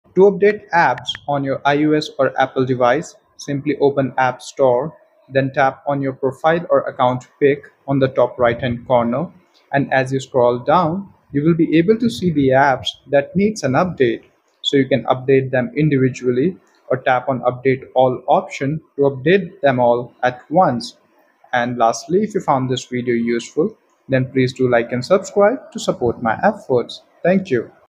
How To Update All Apps On IPhone Using Appstore In Single Click | Update App on iOS
To update apps on your IPhone simply open appstore and then tap on your profile pic on the top right hand corner and then scroll down to find the list of all the avialable apps for which the updates are available. Tap on update option to update any individual app or tap update all to update all the apps at once.